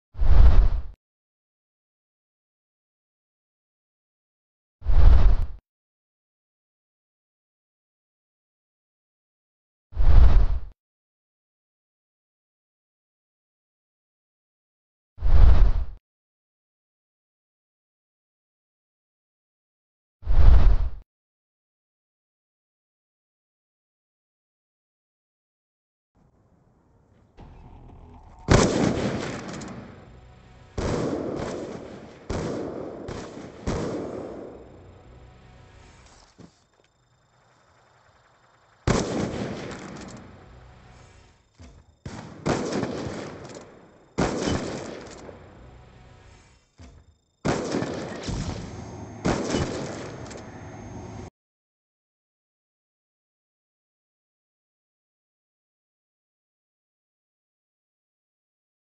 Trickshots Turned into Ground War
Some how that just happened and I hit a crazy Shot at the end watch till end or skip till end LOL